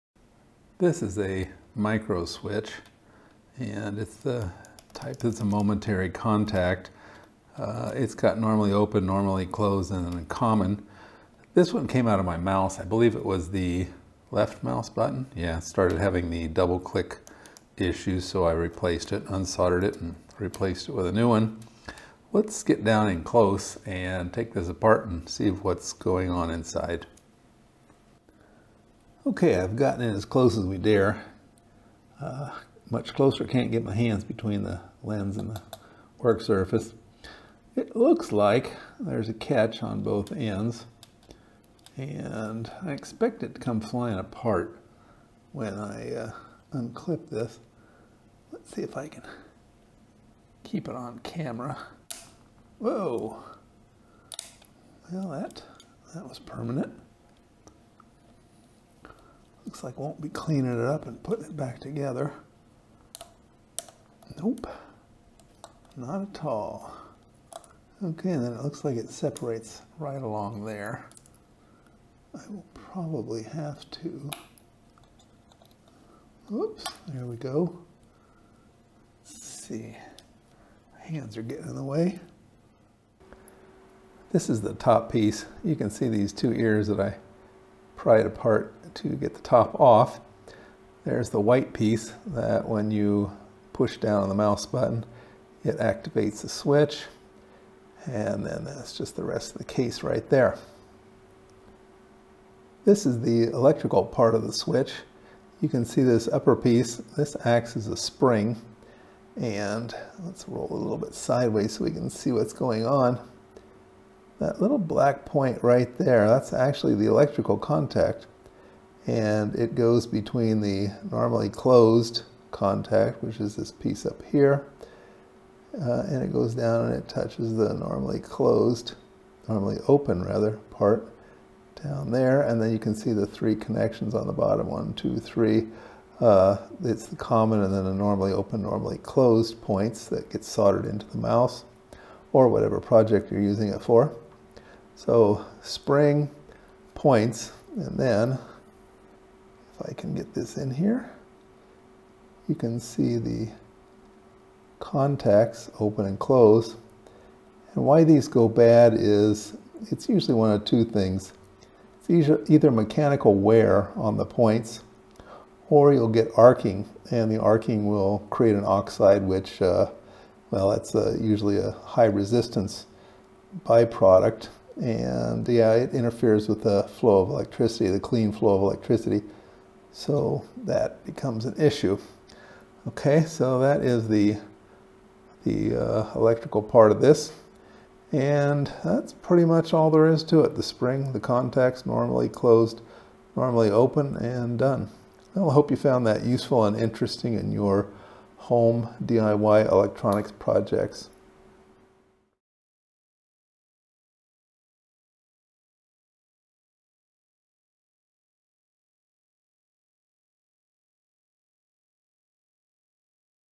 Microswitch Teardown (4K)
Thought we'd tear down a microswitch and see what is inside. This one came out of my expensive mouse. (I replaced its switches twice. Got a video on how to do that.) It was suffering from the dreaded "double click".